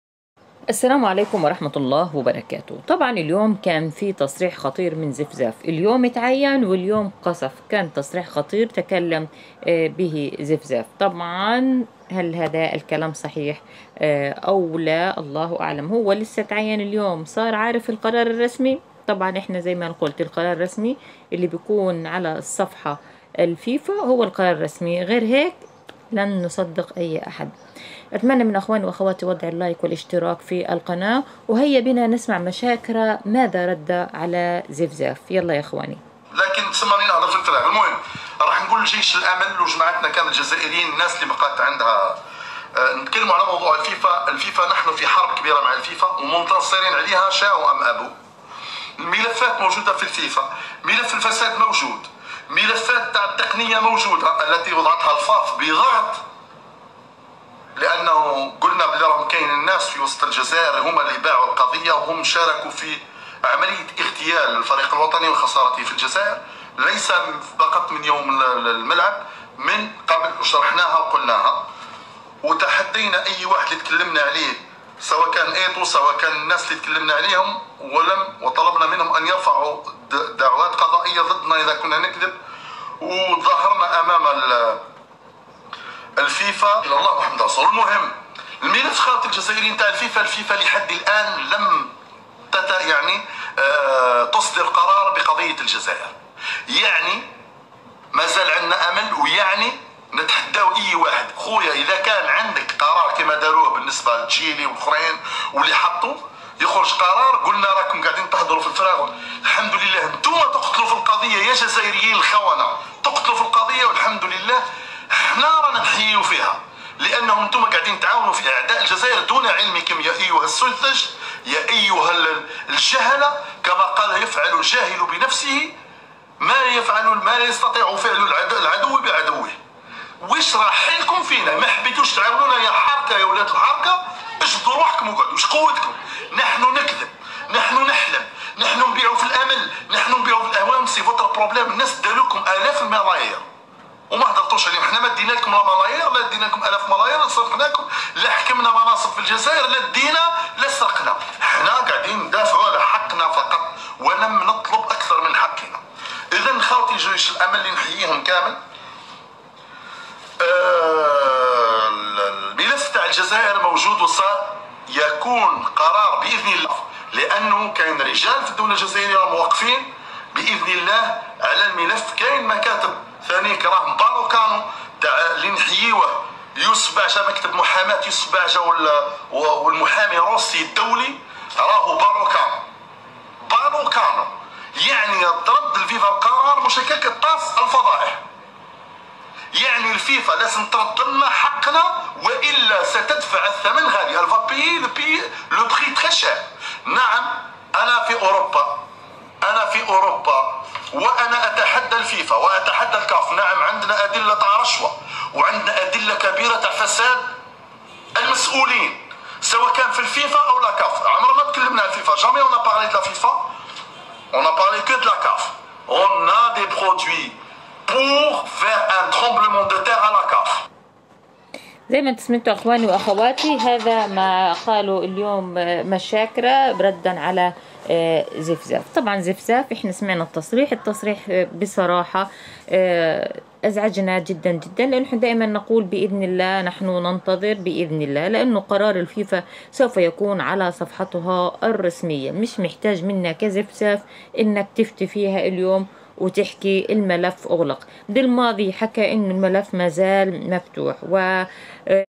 عاجل/ مشاكره يرد وبقوه على زفزاف ويؤكد أن الجزائر بمونديال قطر 2022
يوميات_شاديه_العموري مشاكره يرد وبقوه على زفزاف ويؤكد أن الجزائر بمونديال قطر 2022